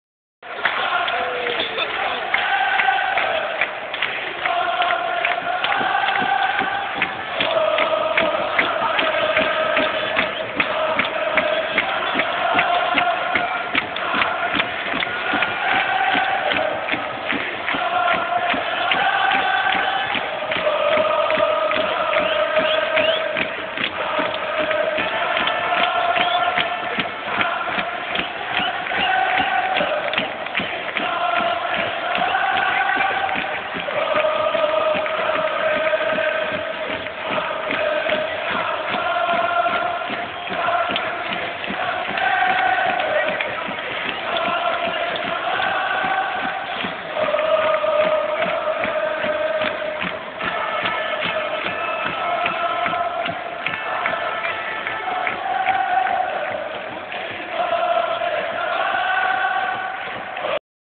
【第２節】浦和×東京（埼スタ） 四面楚歌
納得がいかない敗戦・・・
動画は選手入場時の状況
まさに四面楚歌